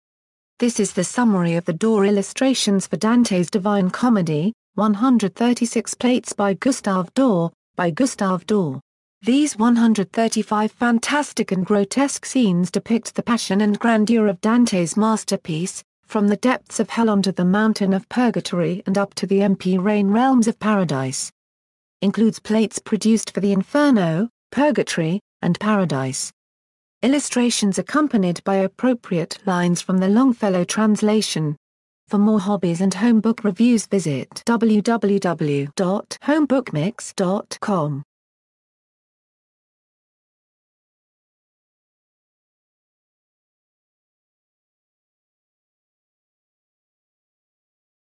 Home Book Summary: The Dore Illustrations for Dantes Divine Comedy (136 Plates by Gustave Dore) ...
This is the summary of The Dore Illustrations for Dantes Divine Comedy (136 Plates by Gustave Dore) by Gustave Dore.